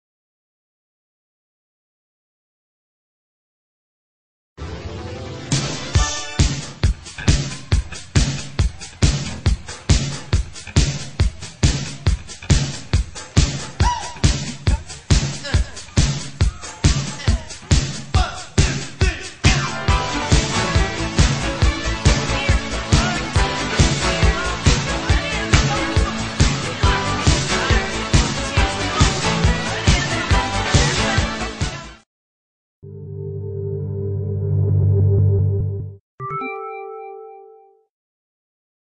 Bottle On Ice TV Commercial ultimate gift bag wine chiller & cooler
Bottle On Ice TV Commercial ultimate gift bag wine chiller cooler ice bucket.
convenient portable foldable reusable durable fun & modern.